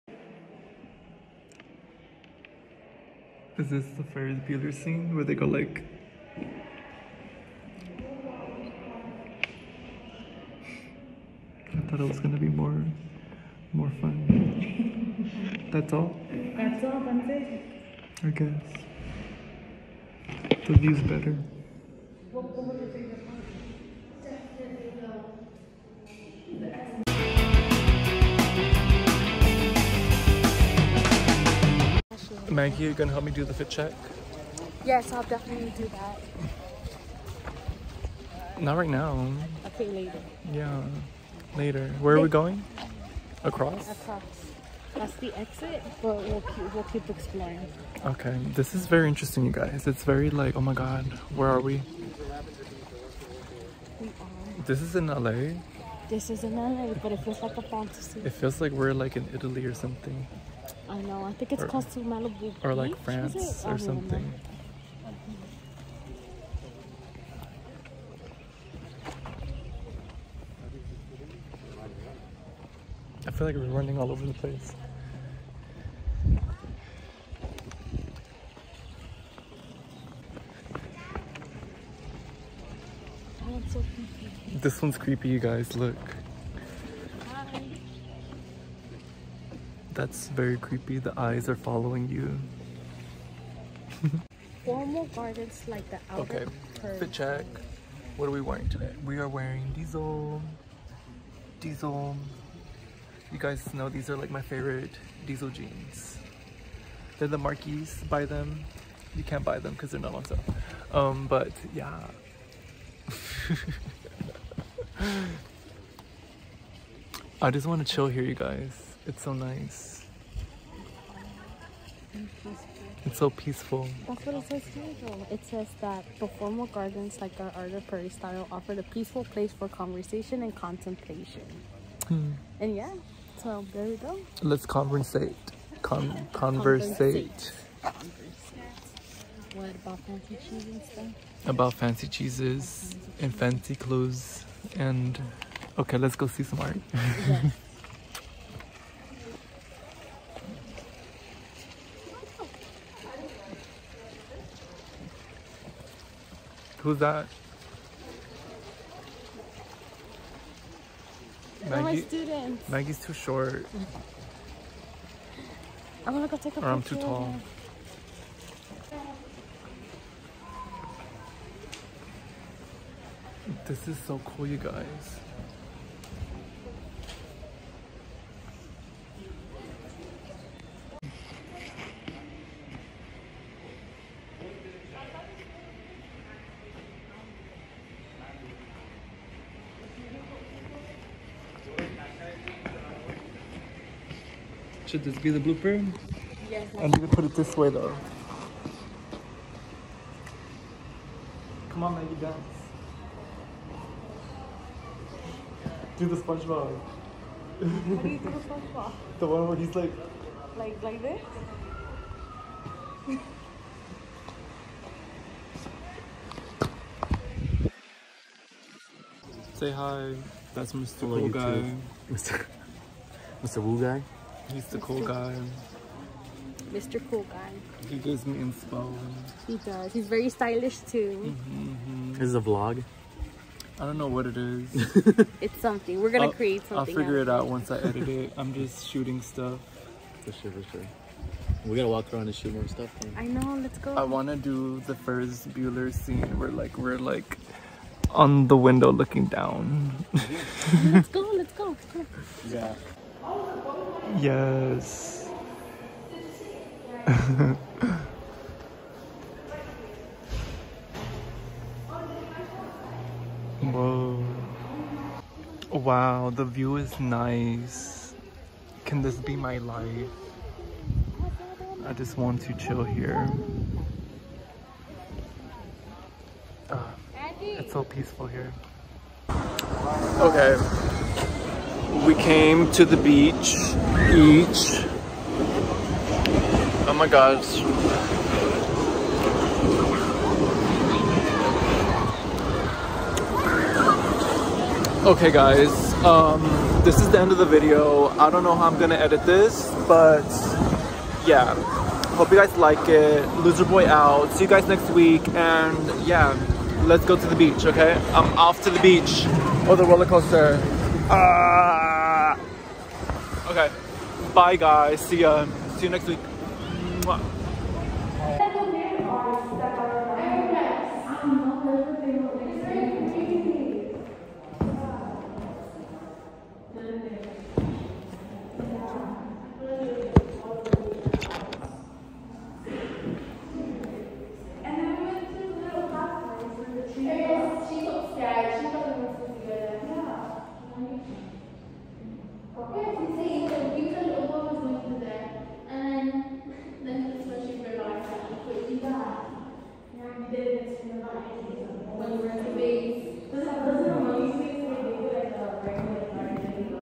the loserboy villa
Not sure what this video is exactly but it's just me experiencing the beauty of this place. Hope you enjoy the video.